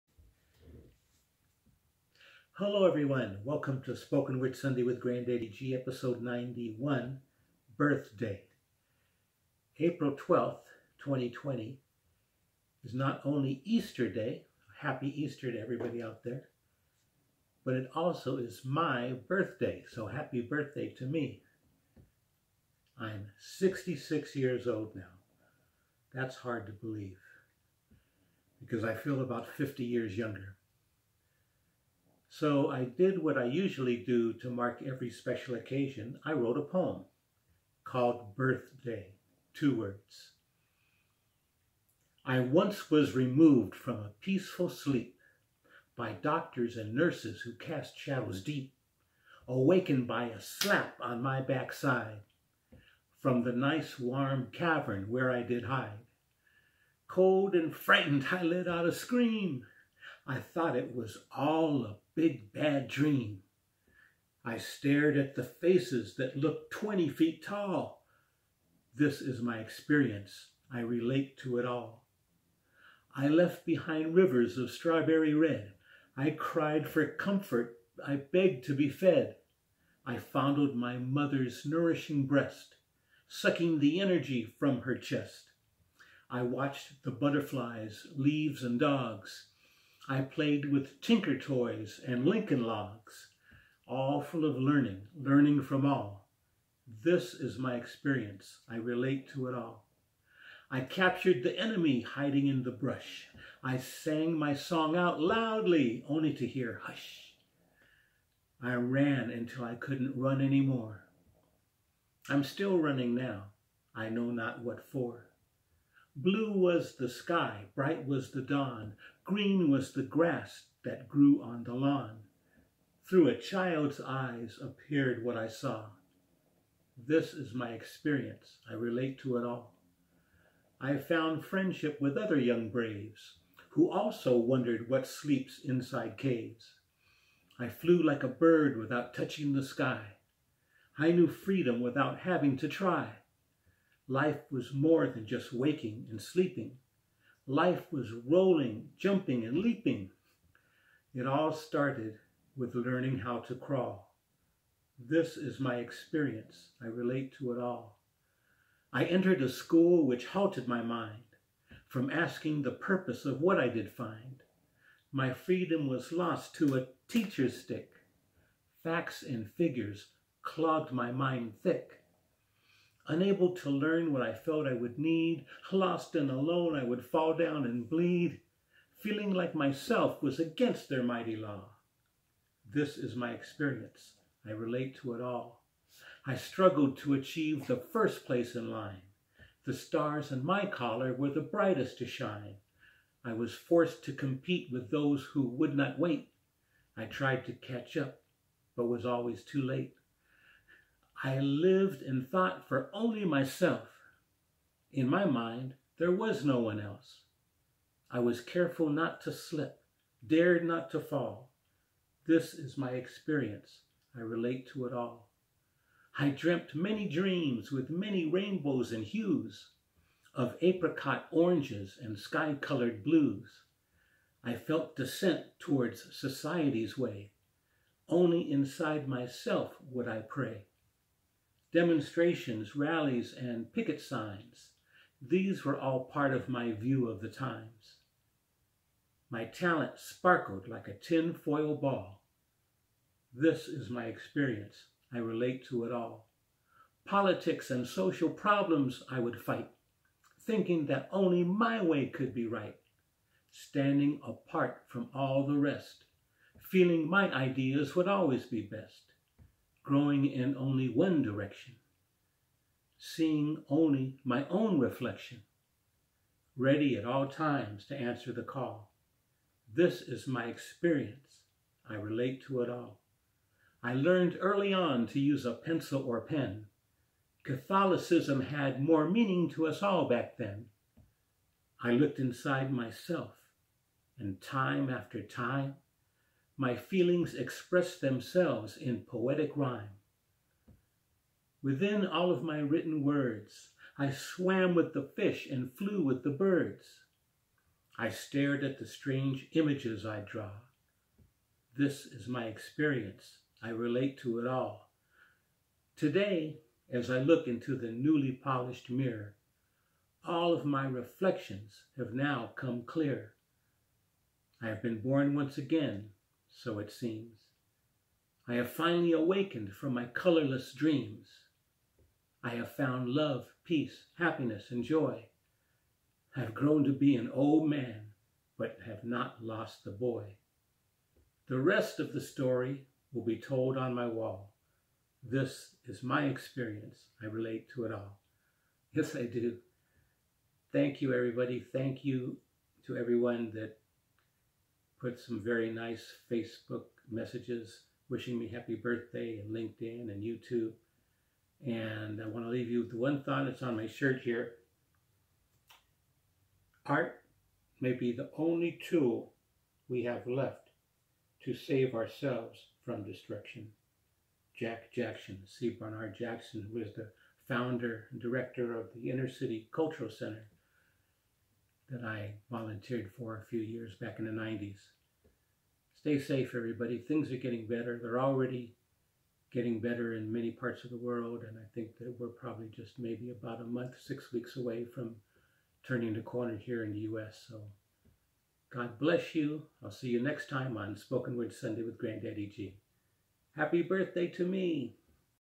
Spoken Word Sunday (Episode 91: Birth Day)
April 12th is Easter Day and my 66th birthday! I wrote this poem to celebrate!
Birth Day by Glenn "Grand Daddy G" Bruno:
I once was removed from a peaceful sleep,
By Doctors and Nurses who cast shadows deep.
Awakened by a slap on my back side,
From the nice warm cavern where I did hide.
Cold and frightened, I let out a scream!
I thought it was all a bad dream.
I stared at the faces that looked twenty feet tall,
This is my experience, I relate to it all.

I left behind rivers of strawberry red,
I cried for comfort, I begged to be fed.
I fondled my mother’s nourishing breast,
Sucking the energy from her chest.
I watched the butterflies, leaves and dogs.
I played with Tinker Toys and Lincoln Logs.
All full of learning, learning from all.

I captured the enemy hiding in the brush.
I sang my song out loudly only to hear “hush”.
I ran until I couldn’t run anymore.
I’m still running now, I know not what for.
Blue was the sky, bright was the dawn.
Green was the grass that grew on the lawn.
Through a child’s eyes appeared what I saw,

I found friendship with other young braves,
Who also wondered what sleeps inside caves.
I flew like a bird without touching the sky.
I knew freedom without having to try.
Life was more than just waking and sleeping,
Life was rolling, jumping and leaping!
It all started with learning how to crawl.

I entered a school which halted my mind,
From asking the purpose of what I did find.
My freedom was lost to a teacher’s stick.
Facts and figures clogged my mind thick.
Unable to learn what I felt I would need,
Lost and alone I would fall down and bleed.
Feeling like myself was against their mighty law.

I struggled to achieve the first place in line.
The stars on my collar were the brightest to shine.
I was forced to compete with those who would not wait.
I tried to catch up but was always too late.
I lived and thought for only myself.
In my mind, there was no one else.
I was careful not to slip, dared not to fall.

I dreamt many dreams with many rainbows and hues,
Of apricot oranges and sky colored blues.
I felt dissent toward society’s way,
Only inside myself would I pray.
Demonstrations, rallies and picket signs,
These were all part of my view of the times.
My talent sparkled like a tin foil ball,

Politics and social problems I would fight,
Thinking that only my way could be right.
Standing apart from all of the rest,
Feeling my ideas would always be best.
Growing in only one direction.
Seeing only my own reflection.
Ready at all times to answer the call,

I learned early on to use a pencil or pen.
Catholicism had more meaning to us all back then.
I looked inside myself and time after time,
My feelings expressed themselves in poetic rhyme.
Within all of my written words,
I swam with the fish and flew with the birds.
I stared at the strange images I’d draw.

Today as I look into the newly polished mirror,
All of my reflections have now come clear.
I have been born once again, so it seems.
I have finally awakened from my colorless dreams.
I have found Love, Peace, Happiness and Joy!
I’ve grown to be an old man but have not lost the boy!
The rest of the story will be told on my wall,